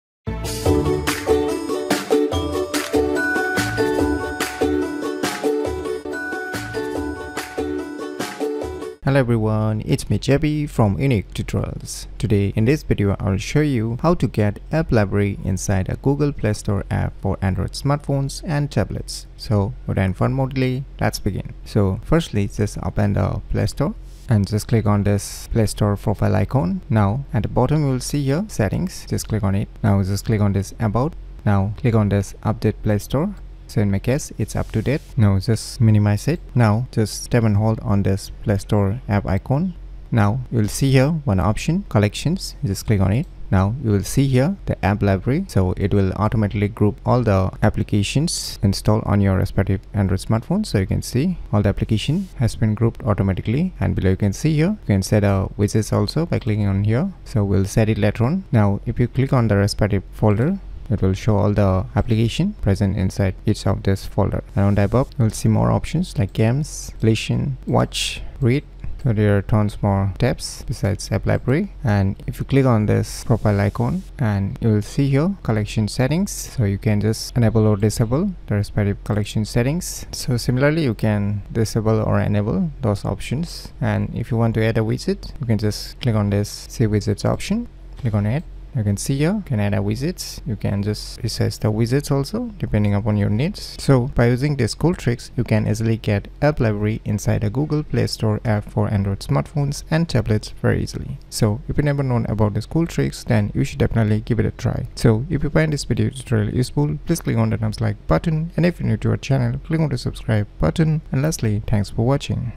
How to get " App Library " inside a Google Playstore app for Android smartphones and tablets ?
This video tutorial is all about How to get " App Library " inside a Google Playstore app for Android smartphones and tablets.